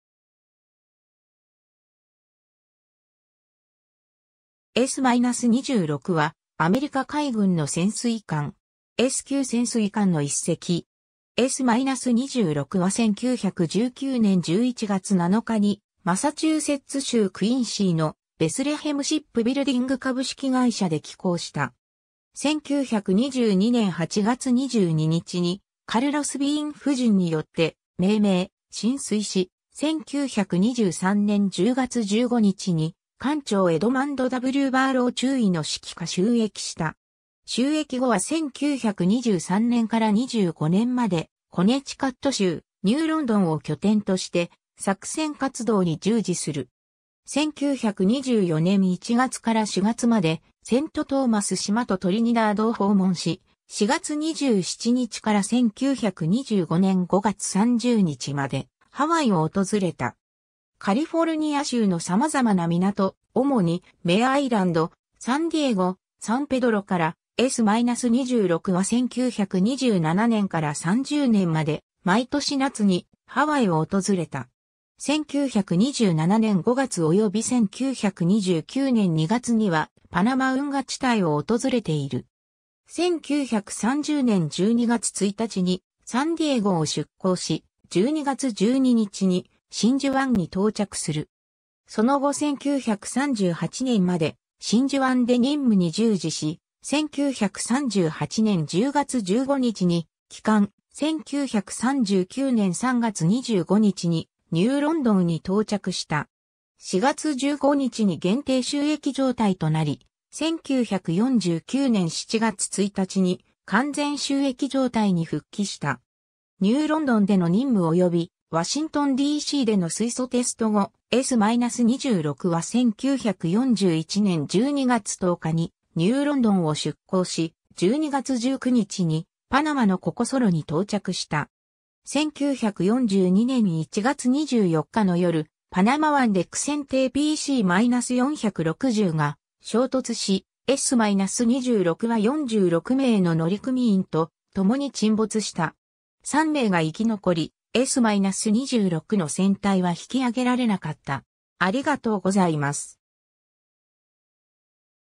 S-26 (潜水艦)
S級潜水艦_(アメリカ海軍)
S-26 (USS S-26, SS-131)は、アメリカ海軍の潜水艦。
S級潜水艦の1隻。
 S-26は1919年11月7日にマサチューセッツ州クインシーのベスレヘム・シップビルディング株式会社で起工した。
1922年8月22日にカルロス・ビーン夫人によって命名、進水し、1923年10月15日に艦長エドマンド・W・バーロウ中尉の指揮下就役した。
 就役後は1923年から25年までコネチカット州ニューロンドンを拠点として作戦活動に従事する。
1924年1月から4月までセント・トーマス島とトリニダードを訪問し、4月27日から1925年5月30日までハワイを訪れた。
カリフォルニア州の様々な港、主にメア・アイランド、サンディエゴ、サンペドロから、S-26は1927年から30年まで毎年夏にハワイを訪れた。
1927年5月および1929年2月にはパナマ運河地帯を訪れている。
1930年12月1日にサンディエゴを出航し、12月12日に真珠湾に到着する。
その後1938年まで真珠湾で任務に従事し、1938年10月15日に帰還、1939年3月25日にニューロンドンに到着した。
4月15日に限定就役状態となり、1949年7月1日に完全就役状態に復帰した。
 ニューロンドンでの任務およびワシントンD.C.での水素テスト後、S-26は1941年12月10日にニューロンドンを出航し、12月19日にパナマのココ・ソロに到着した。
1942年1月24日の夜、パナマ湾で駆潜艇 PC-460 が衝突し、S-26は46名の乗組員と共に沈没した。
3名が生き残り、S-26の船体は引き揚げられなかった。